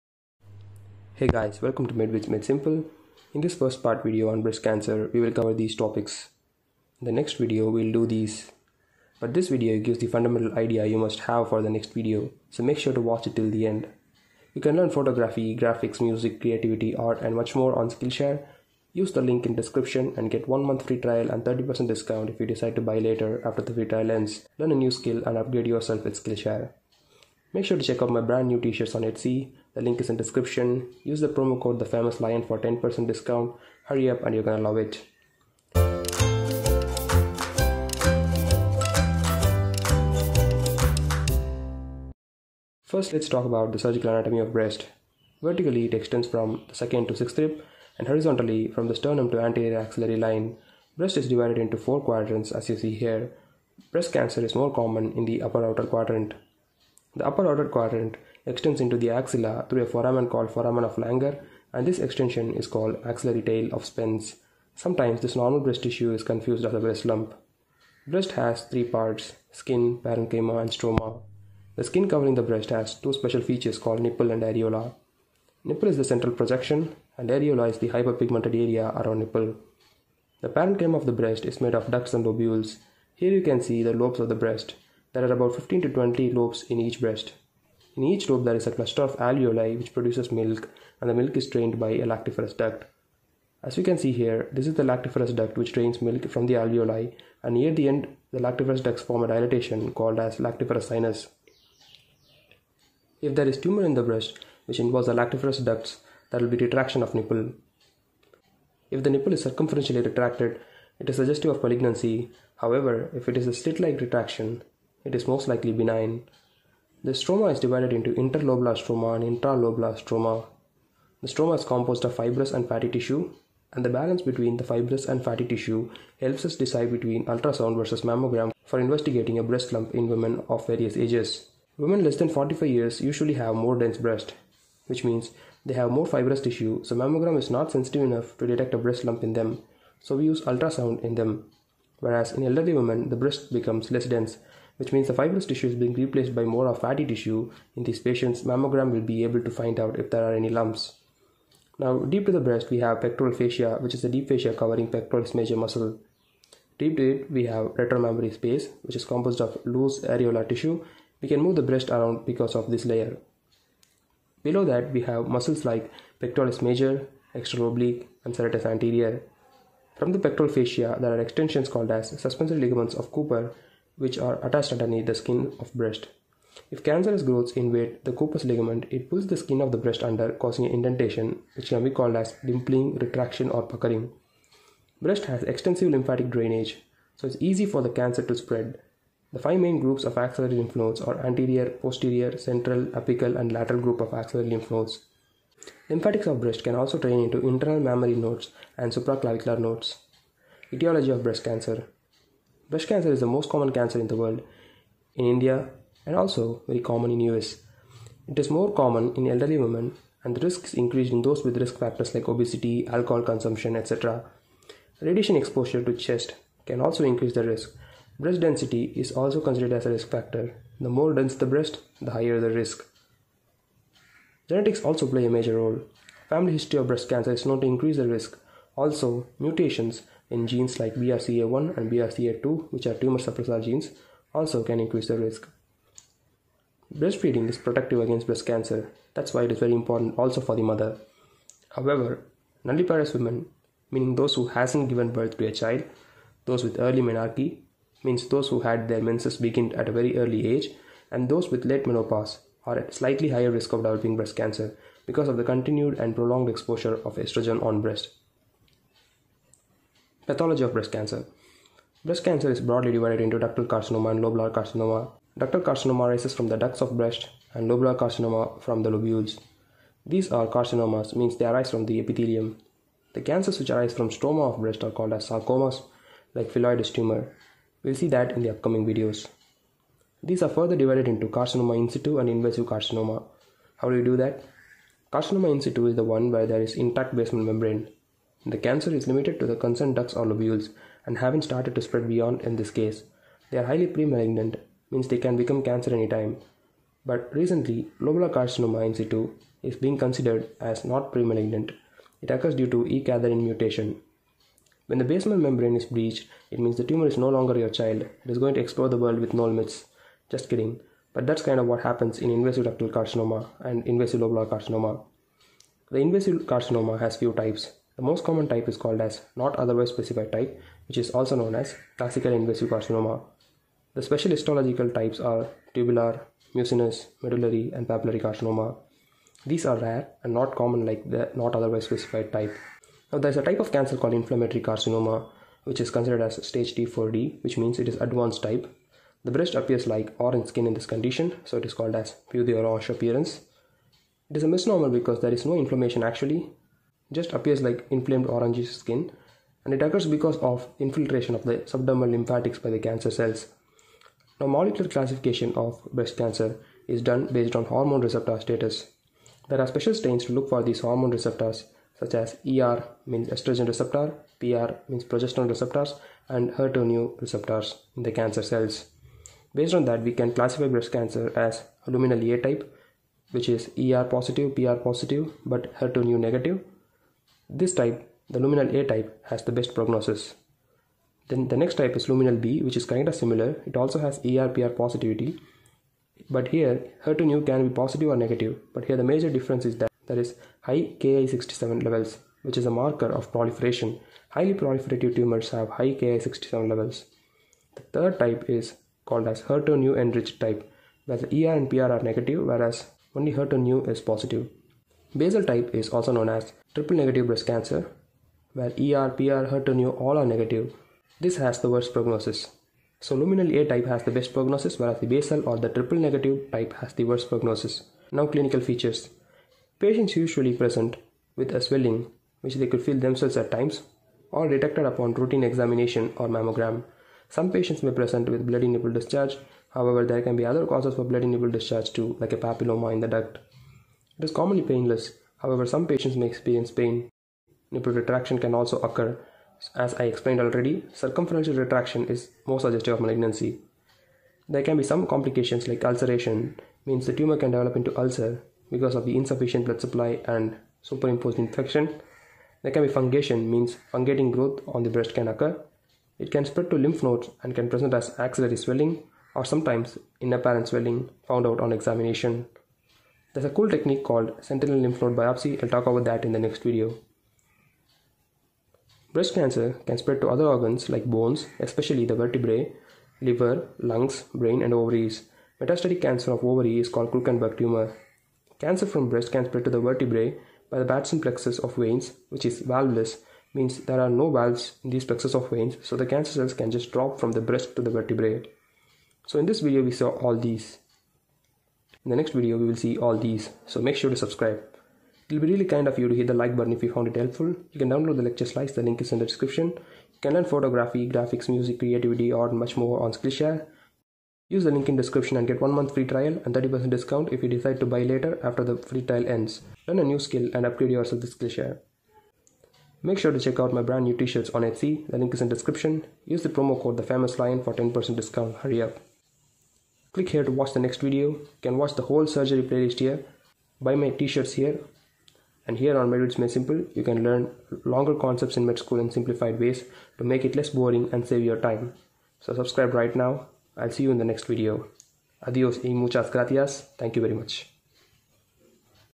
Breast cancer surgery super-simplified - Part 1 (Med Vids Made Simple)
In this video, we will learn about surgical anatomy, etiology, pathology, clinical features, and complications of breast cancer.

Learn photography, art, music, graphic design, creativity, animation, and much more just for one subscription only on SkillShare.

Adiós y muchas gracias. Thank you very much.